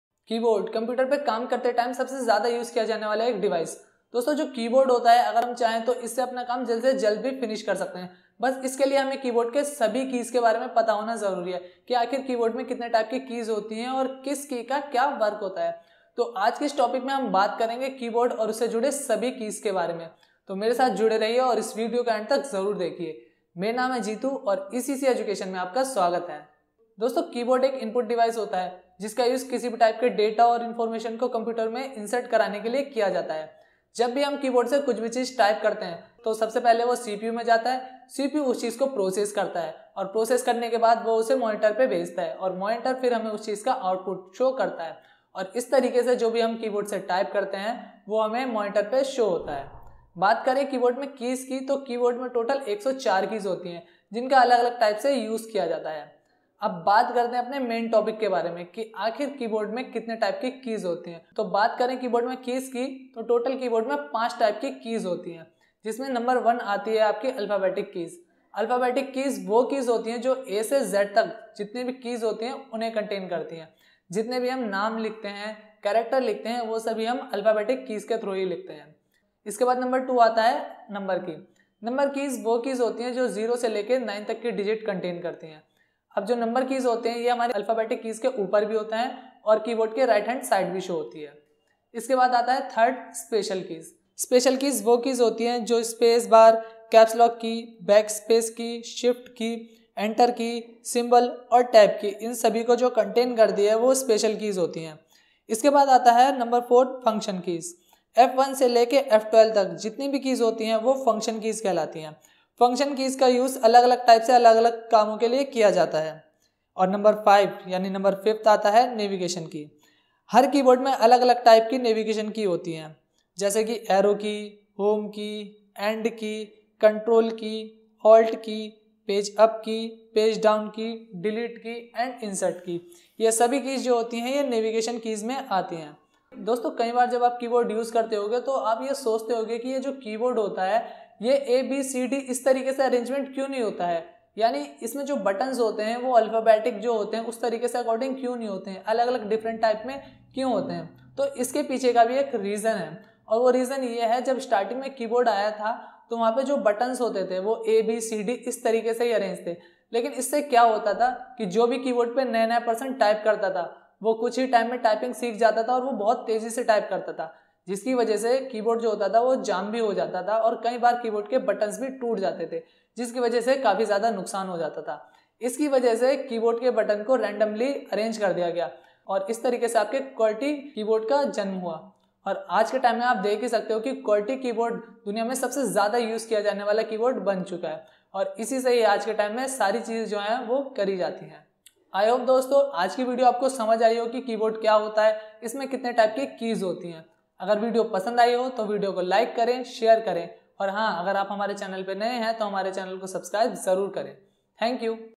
keyboard क्या है? || Types of Keys in Keyboard || Functional | Alphabetic| Navigation etc.
Welcome to ECC Education,
First computer keyboards were adapted from the punch card and teletype equipment. Herman Hollerith developed the first keypunch devices, which soon evolved to include keys for text and number entry akin to normal typewriters by the 1930s. In 1948, the Binac computer (see the lower image) had a Typewriter-Keyboard Unit.
The keys on your keyboard can be divided into several groups based on function:
Typing (alphanumeric) keys. These keys include the same letter, number, punctuation, and symbol keys found on a traditional typewriter.
Alphabetic Keys
Function keys. ...
Navigation keys. ...
Numeric keypad
Special Keys